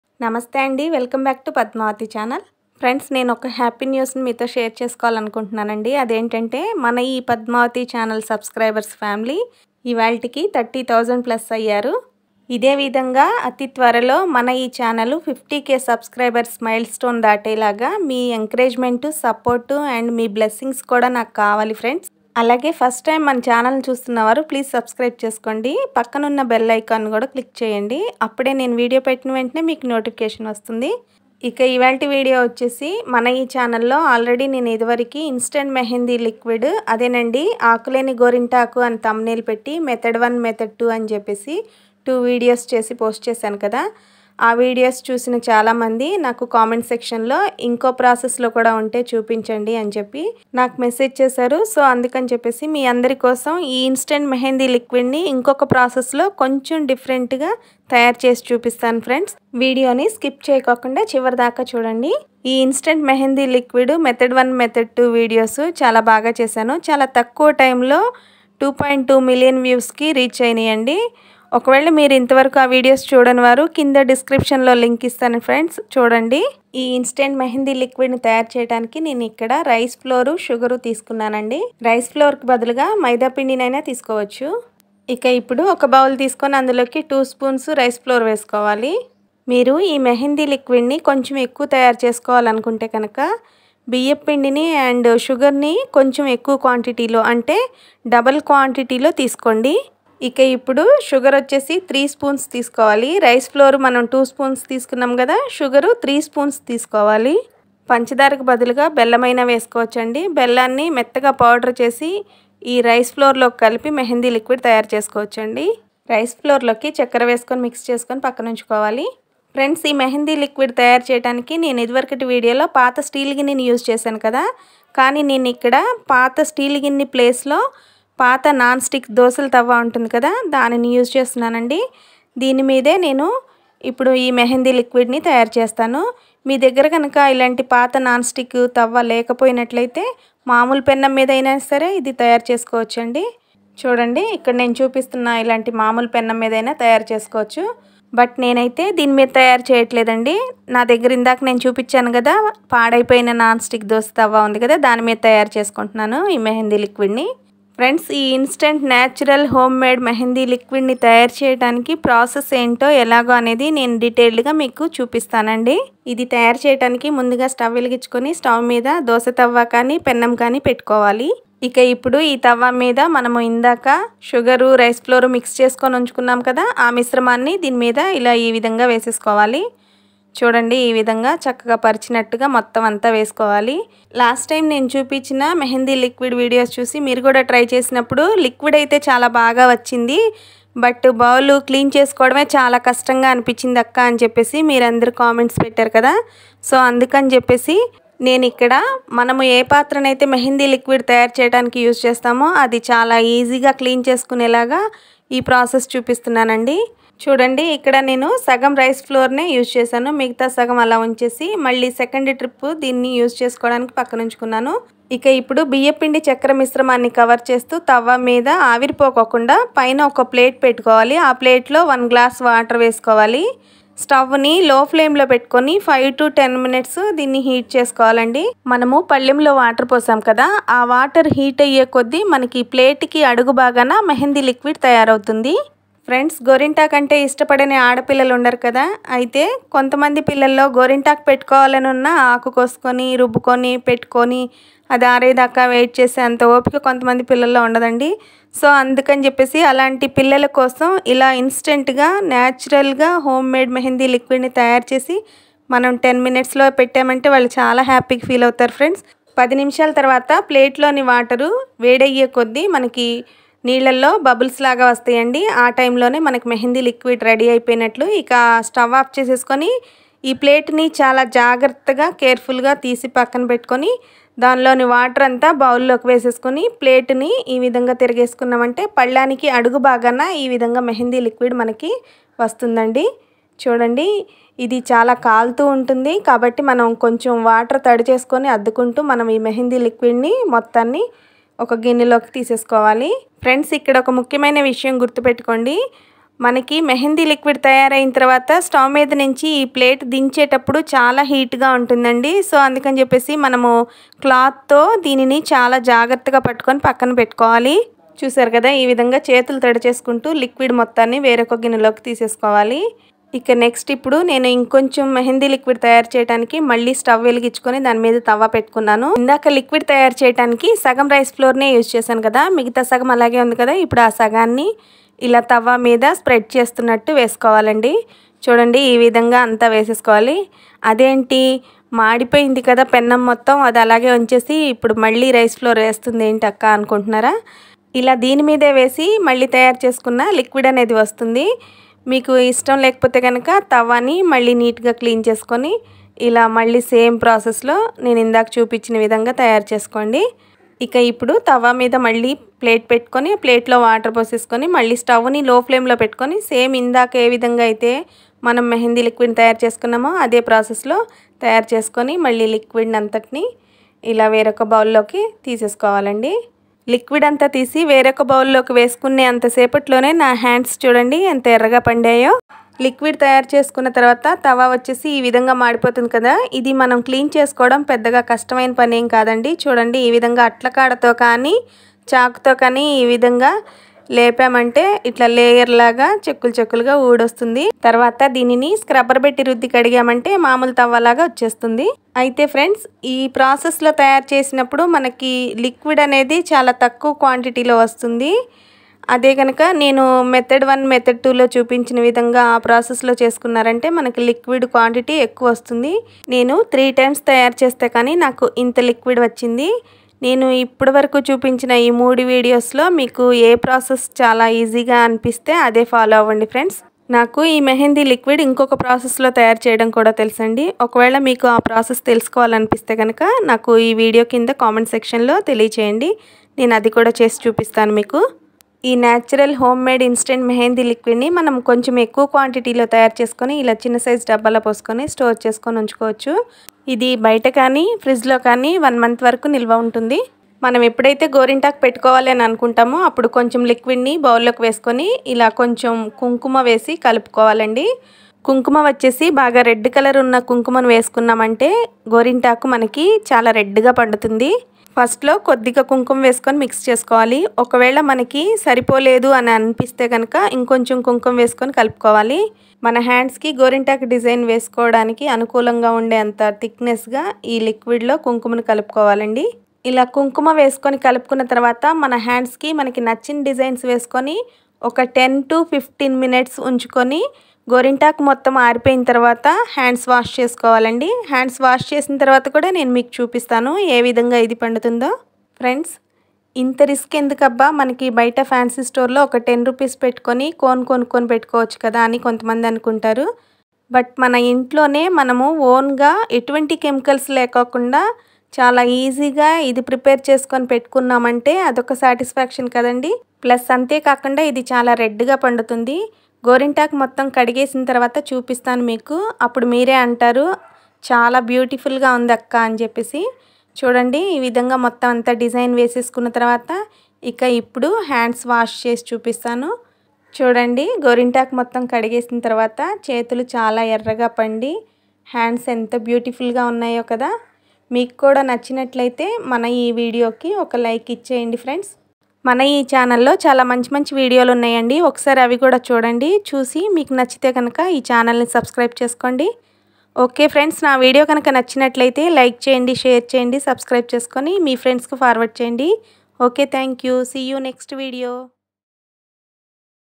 Instant Mehndi liquidTelugu | Instant Organic Mehndi liquid At home | Rice Flour Sugar Mehndi liquid
Instant Mehndi liquid  ( ఆకులేని గోరింటాకు )

Teapowder Mehndi liquid In Telugu

Instant Mehndi liquid Preparation Ingredients : 👇

1. Panchadaara ( Sugar ) - 3 tbsp
2.  Biyyam Pindi ( Rice Flour ) - 2 tbsp
3. Kumkuma ( Kumkum ) - 1/2 tbsp